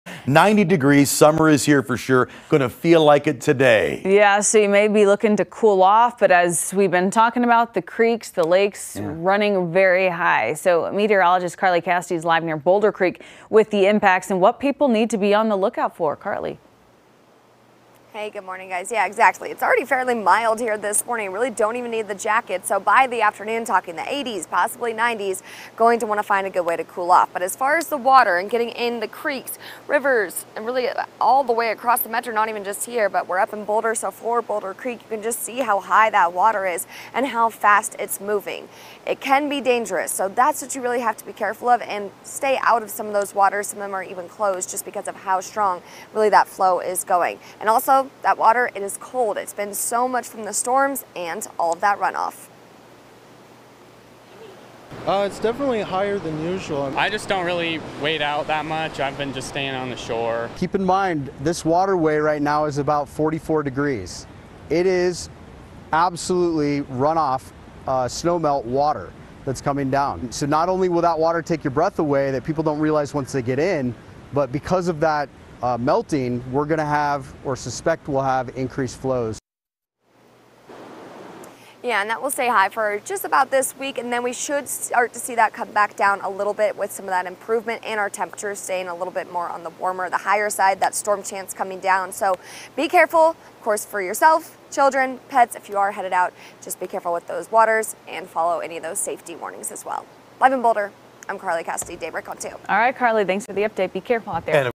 Watch out for high water levels at local creeks and lakes
With temperatures heating up this week, experts are warning recreationists to watch out for fast moving water. Carly Cassady forecasts.